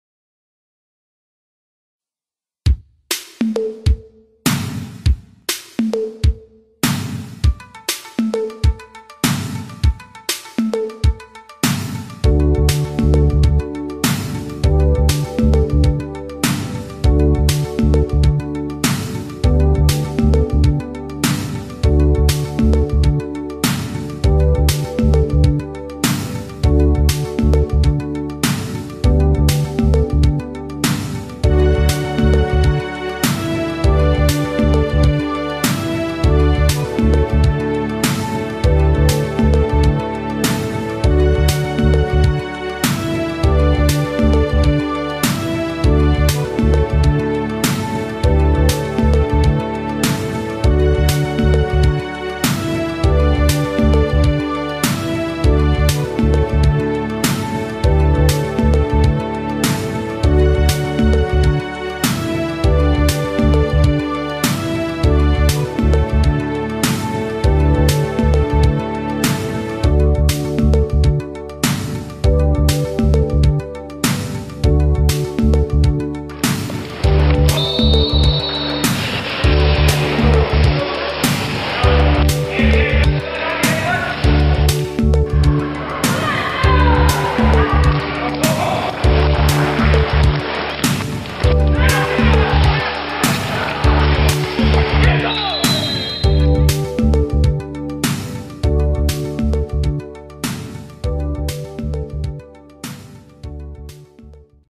Hockey s/Patines | Harrods vs Neuquen
Galeria de Fotos e imagenes en video.
Partido de Liga Nacional A2
Harrods vs Neuquen de Parana
Sabado 17 de septiembre de 2016